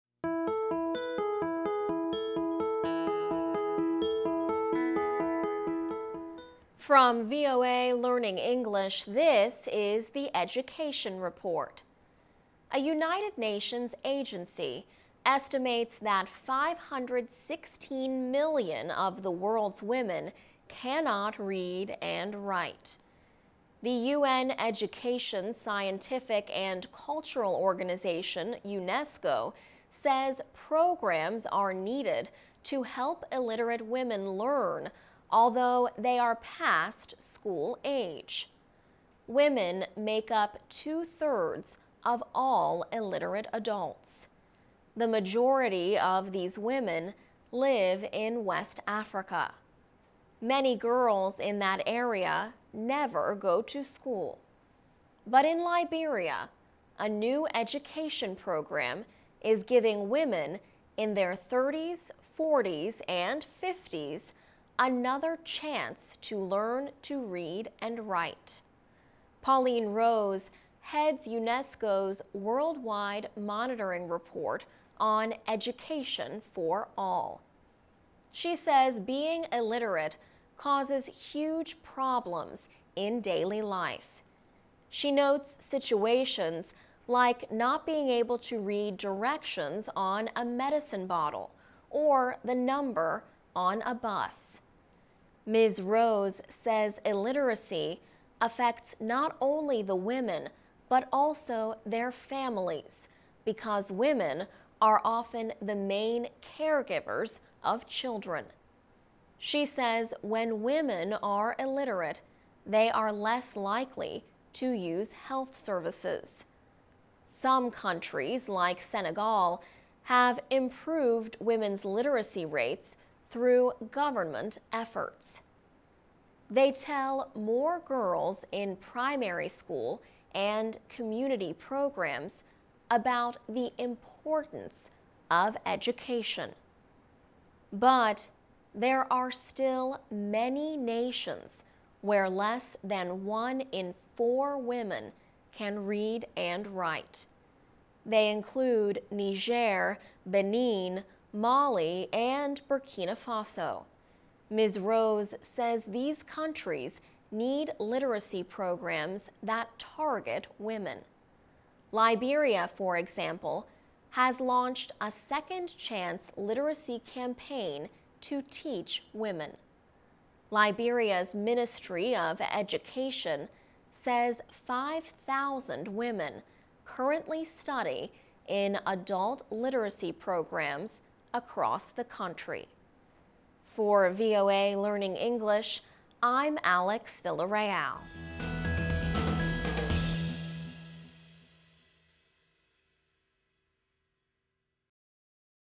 Majority of World's Illiterate Women Live in West Africa
From VOA Learning English, this is the Education Report.

A United Nations agency estimates that 516 million of the world's women cannot read and write. The UN Education, Scientific and Cultural Organization, UNESCO, says programs are needed to help illiterate women learn although they are past school age. Women make up two-thirds of all illiterate adults. The majority of these women live in West Africa. Many girls in that area never go to school. But in Liberia, a new education program is giving women in their 30s, 40s and 50s another chance to learn to read and write. Pauline Rose heads UNESCO's worldwide monitoring report on Education for All.  She says being illiterate causes huge problems in daily life.  She notes situations like not being able to read directions on a medicine bottle or the number on a bus.  Ms. Rose says illiteracy affects not only the women but also their families, because women are often the main caregivers of children.  She says, when women are illiterate, they are less likely to use health services. Some countries, like Senegal, have improved women's literacy rates through government efforts.  They tell more girls in primary school and community programs about the importance of education.  But there are still many nations where less than one in four women can read and write. They include Niger, Benin, Mali and Burkina Faso. Ms. Rose says these countries need literacy programs that target women.  Liberia, for example, has launched a second-chance literacy campaign to teach women. Liberia's Ministry of Education says 5,000 women currently study in adult literacy programs across the country. For VOA Learning English, I'm Alex Villarreal.